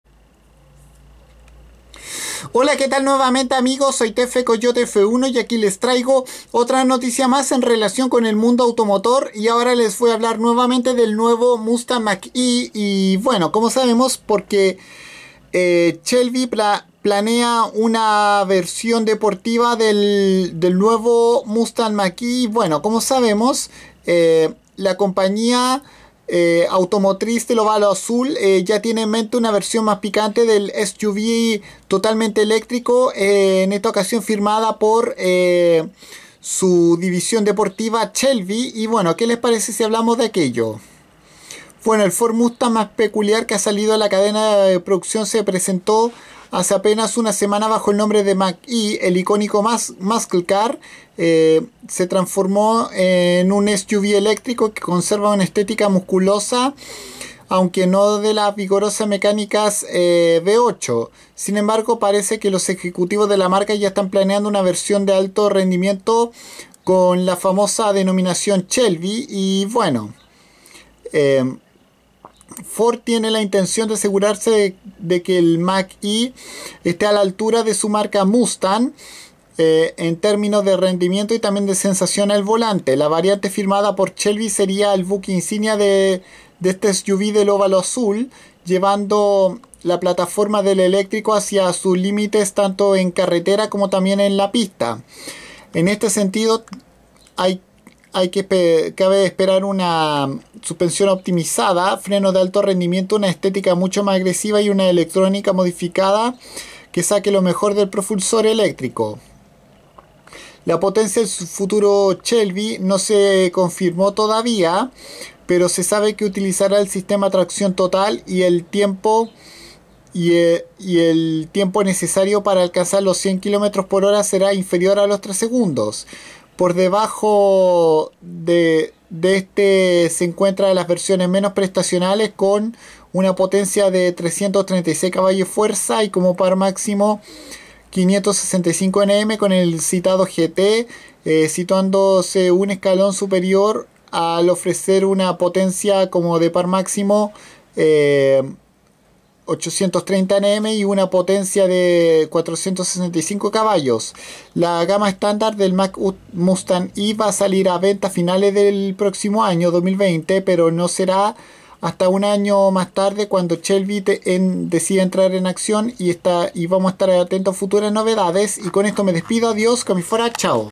Shelby anuncia una versión deportiva del Mustang Mach-E
El nuevo SUV totalmente eléctrico de Ford inspirado en su icónico "pony car" podría tener una versión firmada por Shelby